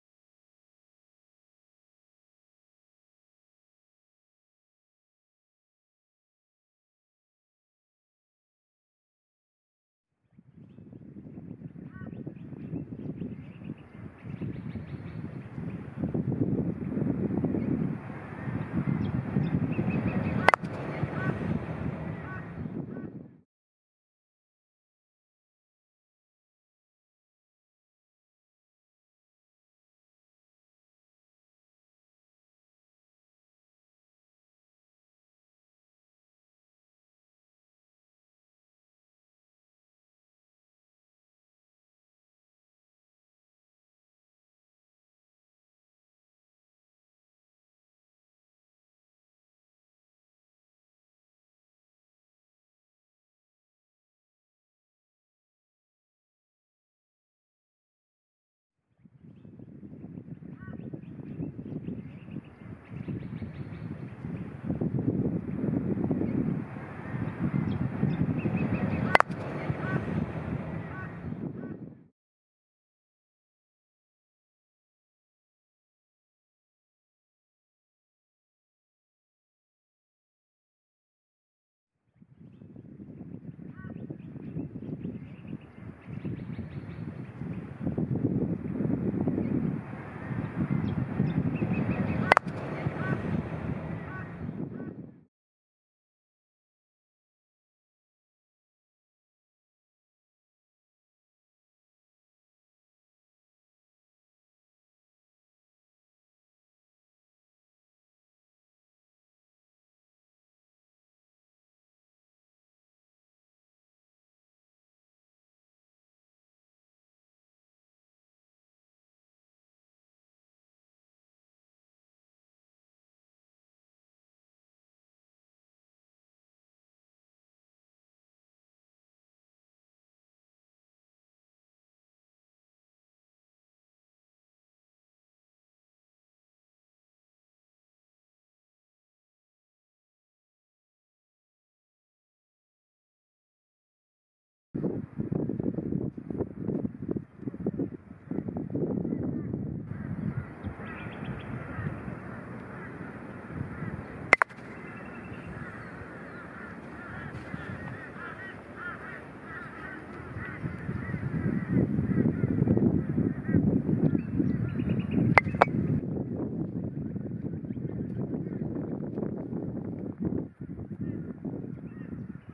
Basic Golf Croquet Stop Shot Tutorial
Simple explanation of a croquet stop shot to help new players.
An essential part of the game that requires understanding and practice.